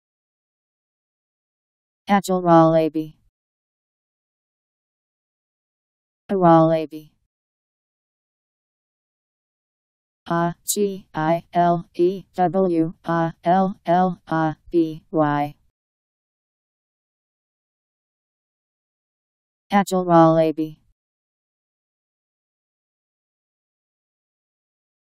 Agile wallaby Meaning
Video shows what agile wallaby means. A wallaby, ..  Agile wallaby Meaning. How to pronounce, definition audio dictionary. How to say agile wallaby. Powered by MaryTTS, Wiktionary